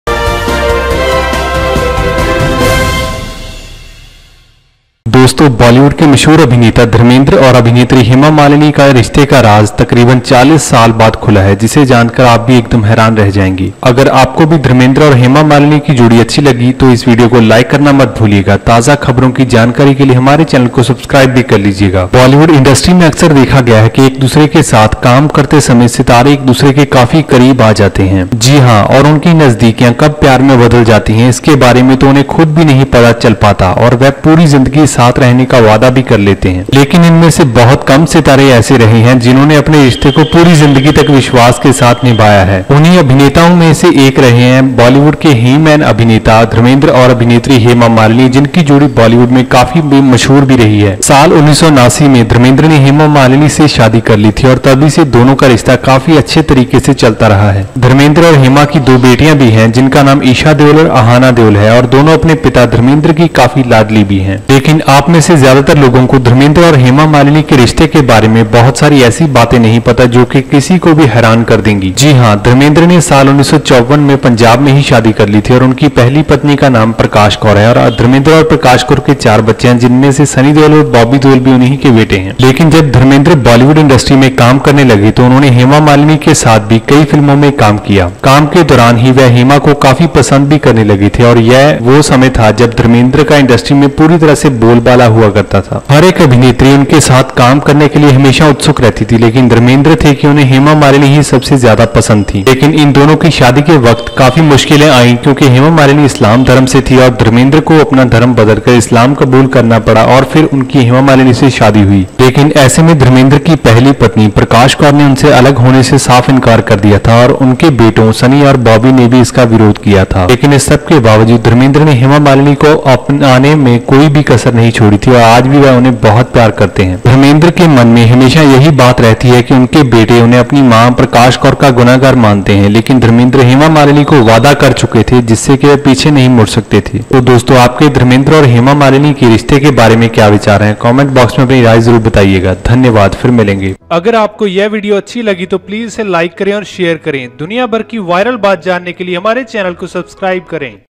40 साल बाद खुला धर्मेंद्र और हेमा मालिनी के रिश्ते का राज़, जानकर चौंक जायेंगे - Dharmendra Hema
40 साल बाद खुला धर्मेंद्र और हेमा मालिनी के रिश्ते का राज़, जानकर चौंक जायेंगे - Dharmendra Hema